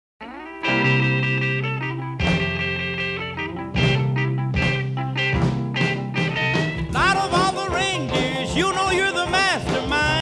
Bringing My Pup Christmas Shopping with His Cute Pajamas
It’s just that time of year! Doesn’t Rio look the cutest in his Christmas pajamas? 🥰 🐶
Our Christmas shopping consists of going to multiple pet stores and waiting in the car for store pickups. Most of the shopping was all for this cutie!

Dog Christmas Pajamas
Dog Booster Seat

FAQ:
How old is Rio? 1 year old
What do you film with? iPhone & G7X
Mini Tripod?
How do you edit?  CapCut